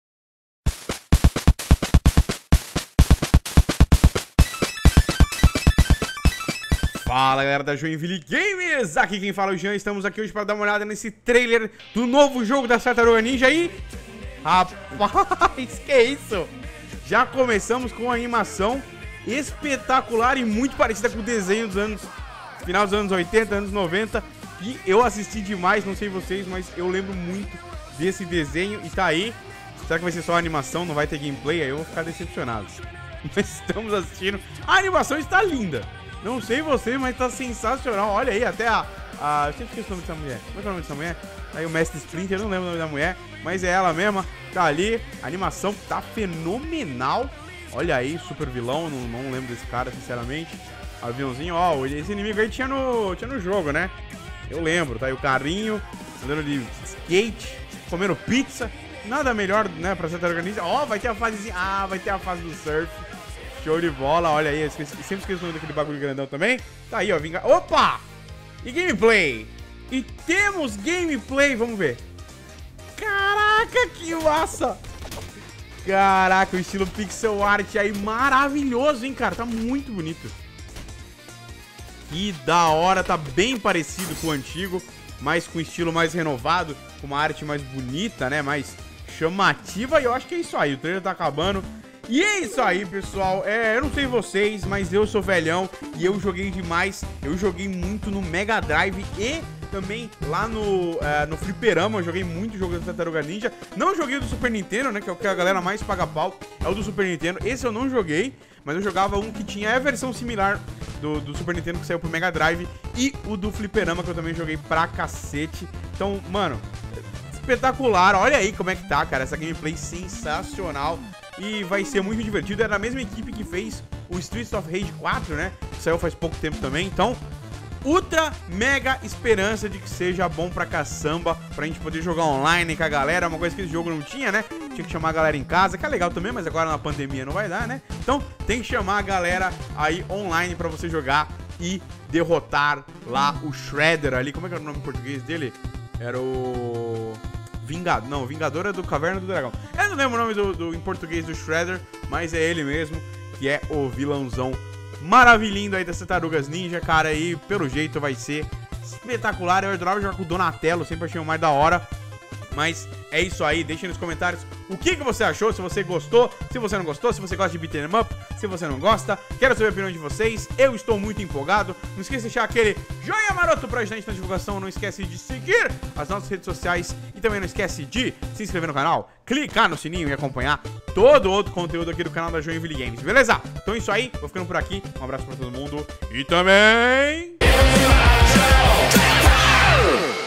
O NOVO jogo das TARTARUGAS NINJA vai ser SENSACIONAL!
Parece que esse jogo novo das Tartarugas Ninja vai ser uma volta aos clássicos da geração Super Nintendo e Mega Drive!

ENTRE EM CONTATO COM A GENTE:

Cadastre agora!